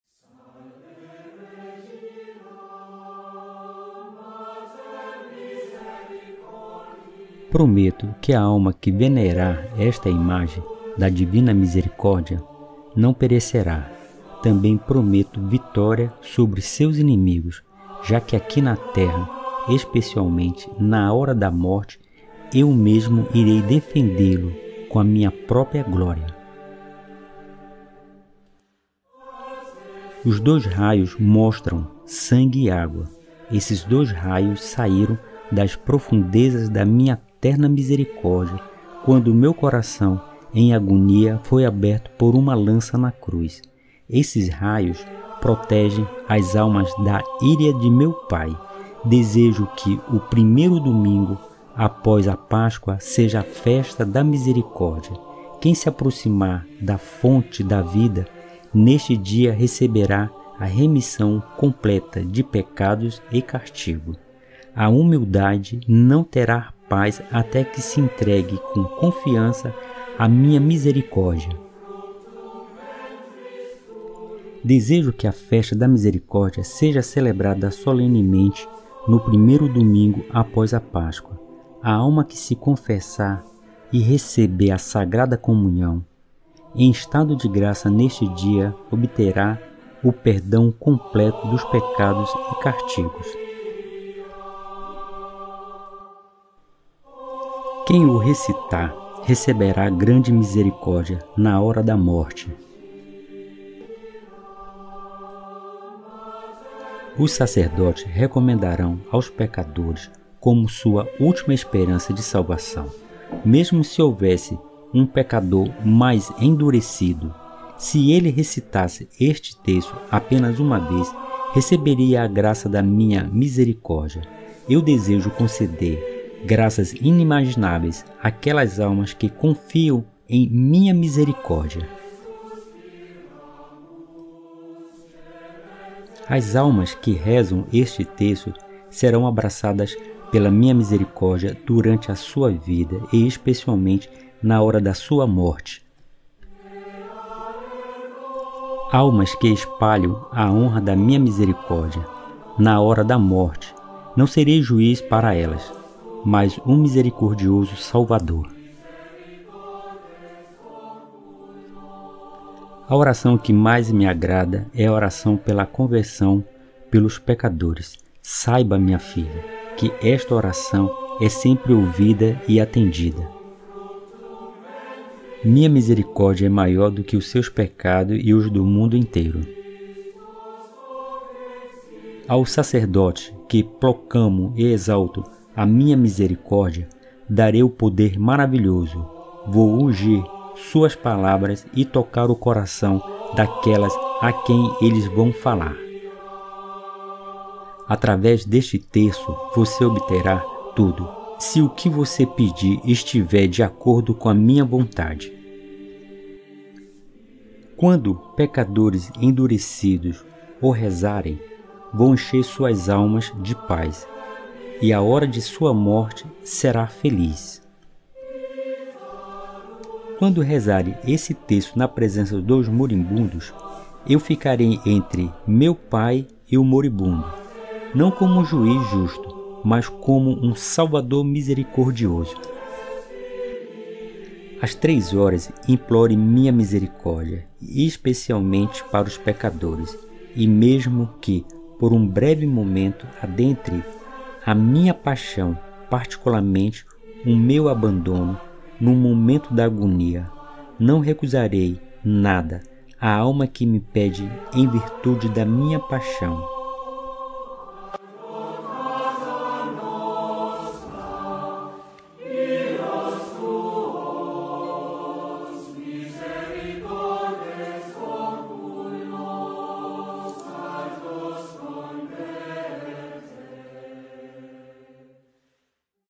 14 promessas que Jesus fez a quem rezar o Terço da Divina Misericórdia
As 14 promessas que Jesus fez a quem rezar o Terço da Divina Misericórdia. Cada promessa irá tocar o seu coração, assista com atenção e passe rezar o  terço da Divina Misericórdia todos os dias as 15 horas .